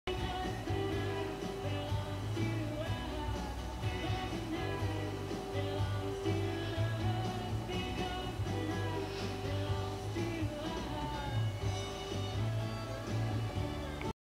MVI 3923 Queensland Australia Koroit Boulder Opal
This opal is from the Stockade mine in Koroit Queensland and is now available on our Opal Auctions Shop -53 Frogs